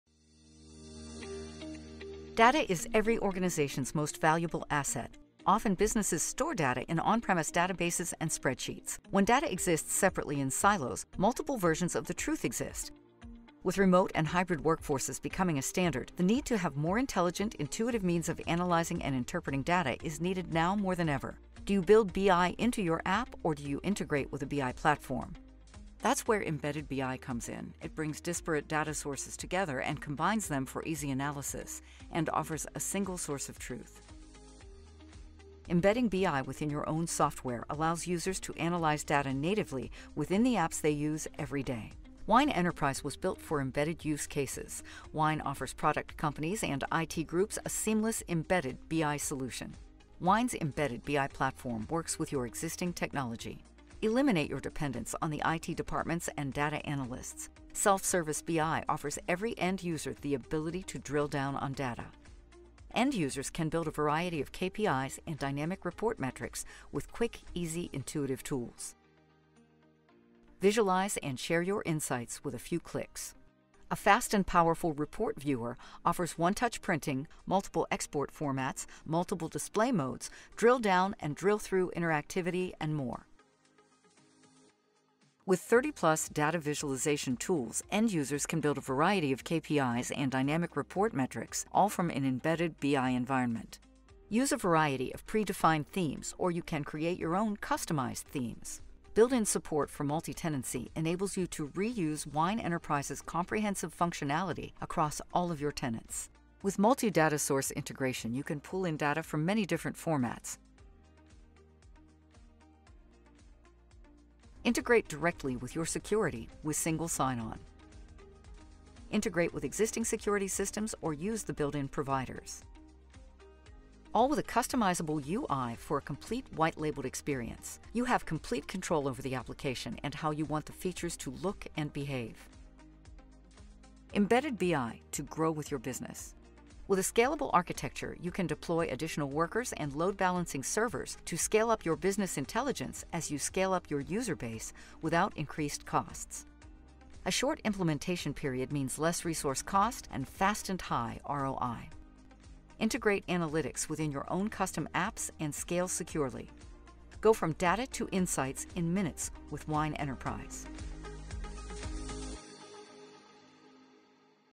Wyn Enterprise | A Scalable Embedded BI Solution
Wyn Enterprise is a complete BI and advanced analytics solution. Wyn's embedded BI platform gives users across any vertical an intuitive portal for interacting with their data. Wyn can be woven into an organization's existing software applications, reducing end-user push-back and minimizing the learning curve.

Designed for self-service BI, Wyn's easy-to-use dashboard and report designers enable users to develop their own ad-hoc dashboards and reports based on the secured data. In the era of post-pandemic, work-from-home, and hybrid workforces, self-service BI provides advanced analytics capabilities to any employee—even if they’re working from the kitchen table.

Self-service BI allows users to run their own queries and create their own reports, even if they don’t have a background in statistical analysis. Since the data is governed and secured, there is no risk of data leaks or data inconsistencies. There is always a single source of the truth.

Discover the story behind your data with Wyn Enterprise. Embed advanced analytics, interactive dashboards, and distributed reporting into any multi-tenant application with no per-user fees or data limitations.

Features:

• Embedded BI
Seamlessly embed analytics within your own applications. Integrate dashboards into your business applications with API and iFrame capabilities.

• End-user BI Reports
Leverage Wyn's powerful and fast reporting engine to develop complex BI reports.

• Interactive BI Dashboards
Analyze data and deliver actionable information with interactive dashboards, multi-dimensional dynamic analysis, and intelligent drilling.

• Easy-to-use Designers
Drag-and-drop controls enable non-technical end-users to visualize, analyze, and distribute permitted data easily.

• Self-service BI
Eliminate your dependence on the IT departments and data analysts. Offer every end-user (from code-first to code-free) the ability to create custom ad hoc reports and interactive dashboards.

• Built-in Multi-tenant Support
Enhance your SaaS-based business applications with a BI platform that natively supports multi-tenancy. Manage your BI resources and data across all your customers from a single environment.

•  Scalable Architecture
Deploy additional workers and load-balancing servers to scale up your business intelligence (as you scale up your user base) without increased costs.

•  White-label Solution
Utilize along with your own custom UI to provide a unique BI experience for your users.

• Exceptional Support
Unlimited support with our supports team, composed of industry technical professionals that understand the model process of data visualization.

Say Goodbye to User Fees

You need a scalable BI platform. You also need to know how much it will cost in the long run. Wyn's scalable licensing model allows room for your business to grow without growth in licensing fees.
• No per-user fees
• No limits on data size

20+ Years in the Industry

Unleash the full potential of embedded ad hoc reporting, driven by the same ActiveReports engine that has led the industry for 20+ years.